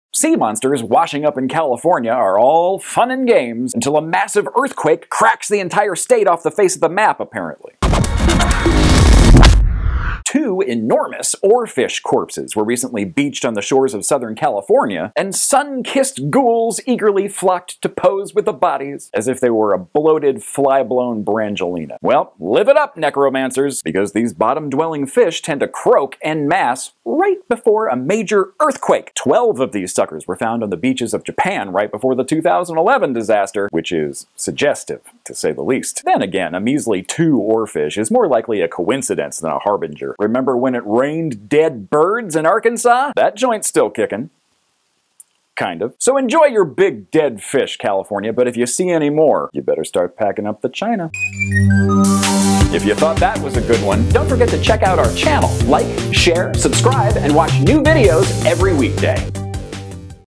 Are All These Dead 'Sea Serpents' a Sign of Impending Disaster?
Seeing one dead oarfish is pretty rare, but southern California just got two.  Is this just coincidence, or a very bad omen?

New Videos from Mackinley every weekday!